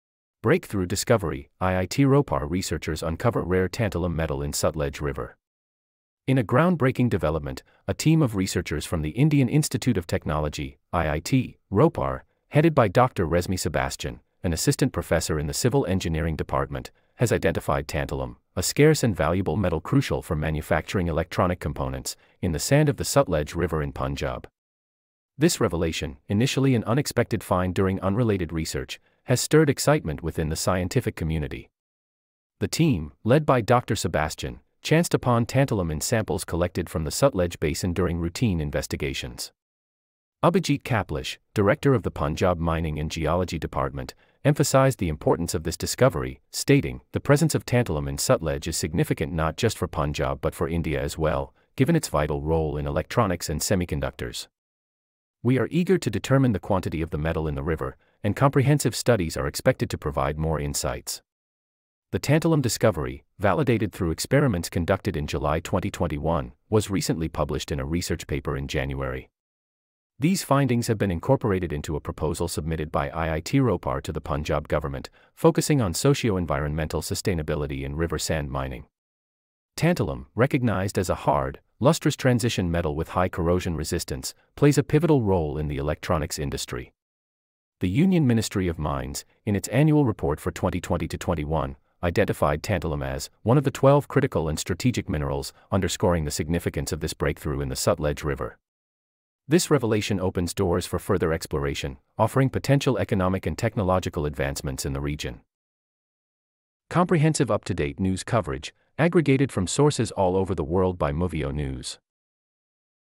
Unveiling the Marvel: Molten Insight Uncovers Rare Tantalum Discovery in Sutlej River!
🔍 Embark on a scientific journey with Molten Insight as we bring you the exclusive revelation of tantalum, a rare and precious metal, discovered in the sands of the Sutlej River. Join the exploration led by Dr Resmi Sebastian and the dedicated team from the Indian Institute of Technology (IIT), Ropar, as we delve into the potential implications for the electronics and semiconductor industries.